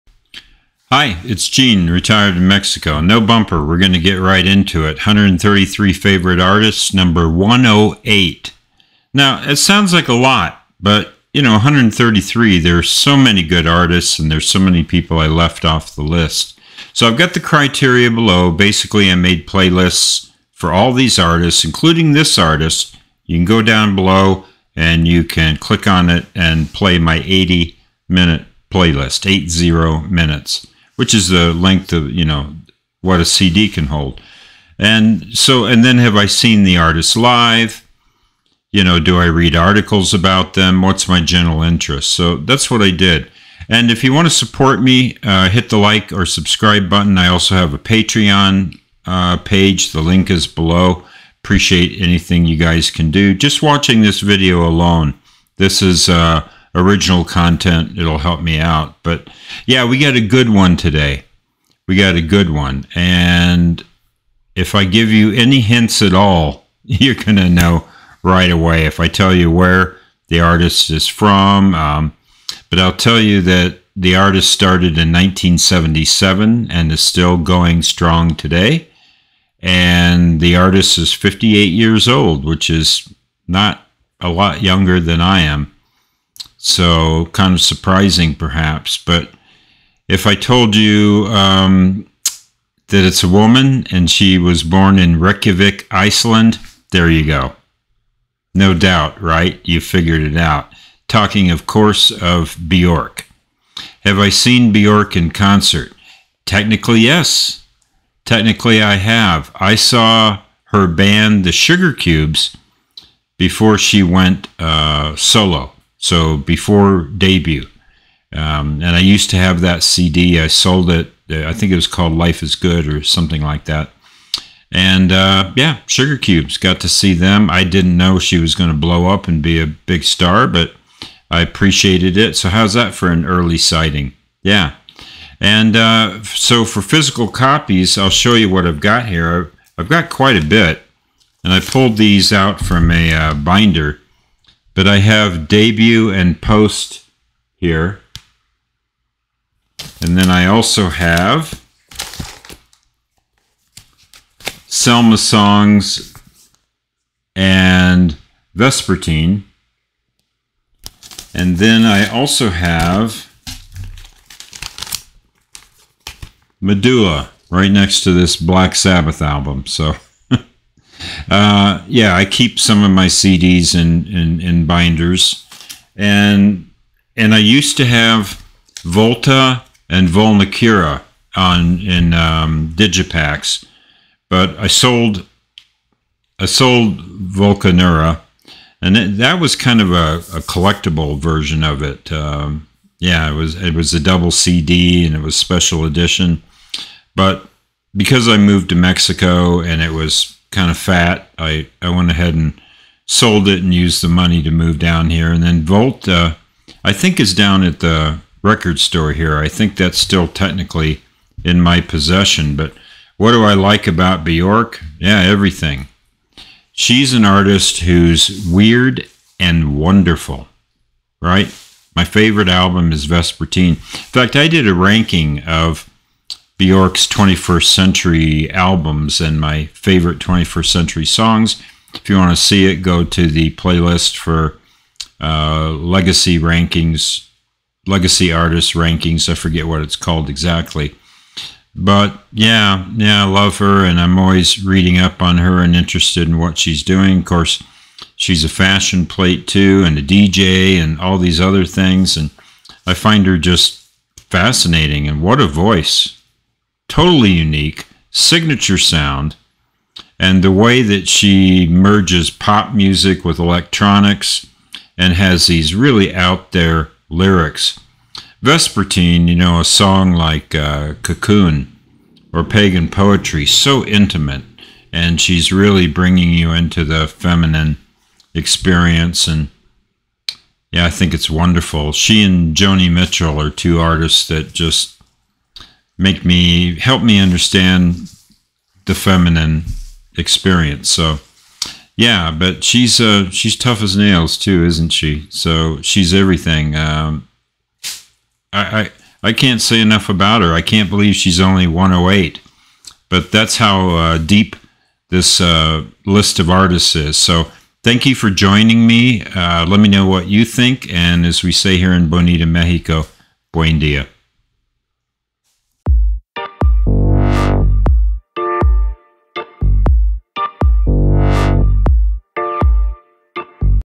133 Favorite Artists #108
New series of my favorite groups and artists.  Criteria: 1. 80-minute playlist all killer no filler 2. Do I read articles or books? 3. What is my live/concert experience?